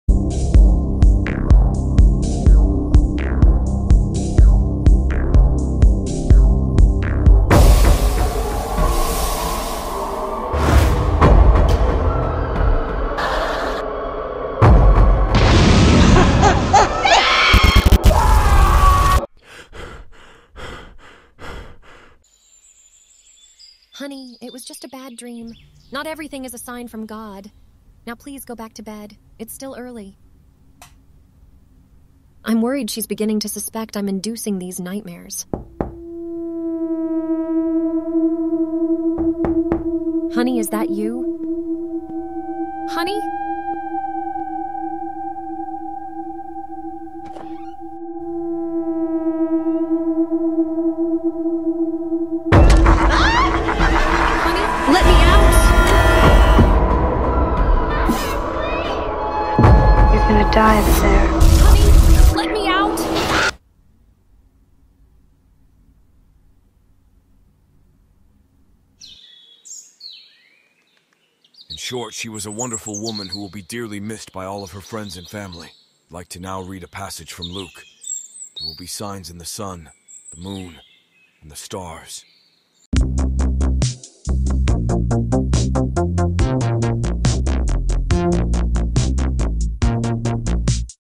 “A￼ Sign￼” AI short film
Made for @Pika_Labs daily entry. Theme was ￼Sign￼s. I￼t’s not easy to make a short in a day with original music, etc & still attend to life + two sick kids. 🤪 But it was fun!